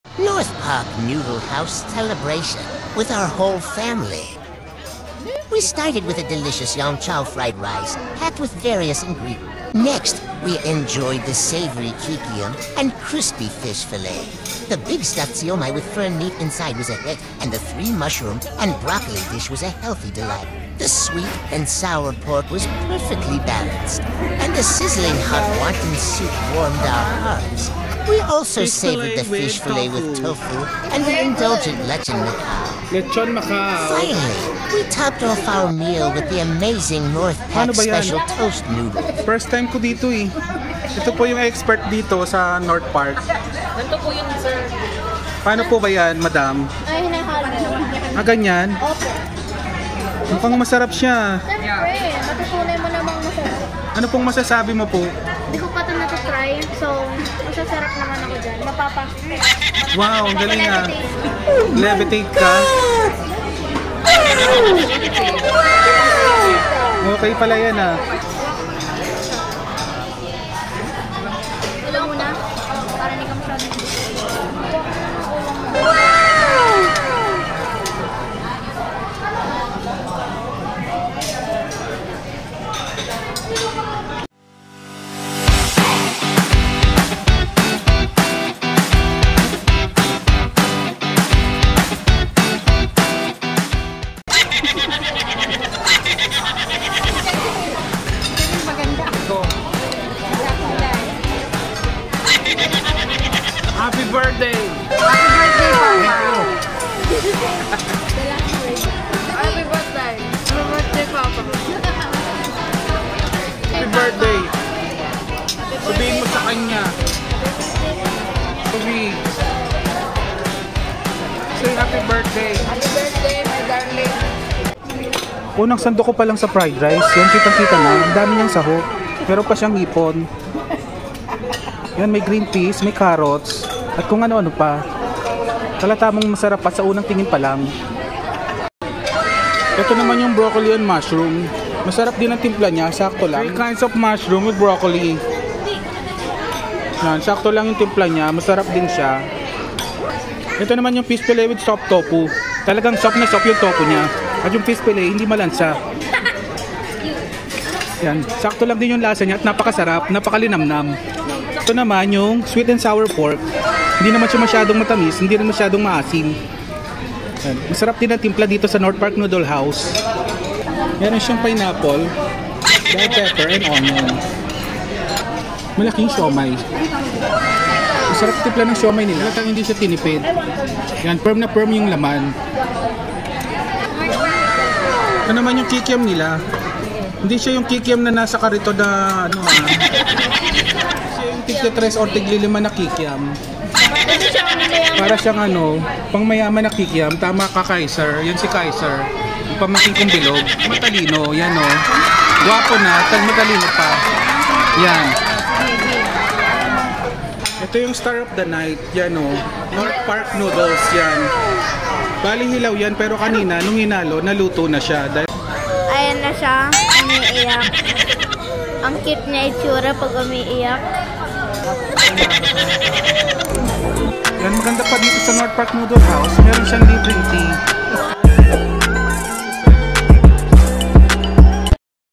"North Park Noodle House Celebration: We Never Knew It Was This Good! reels
oin us for a heartwarming celebration of our father's birthday at North Park Noodle House! We never knew North Park Noodle House was this good until we indulged in their delicious dishes. From the flavorful Yang Chow Fried Rice, savory kikiam, and crispy fish fillet to the big stuffed siomai with firm meat inside, every bite was a delight. We also enjoyed the '3 Mushroom and Broccoli' dish, sweet and sour pork, sizzling hot wanton soup, fish fillet with tofu, and the indulgent Lechon Macau. To top it all off, we savored the AMAZING NORTH PAЯK Special Toast Noodles. Don't miss out on this culinary journey with our family! 🎂🥢🍜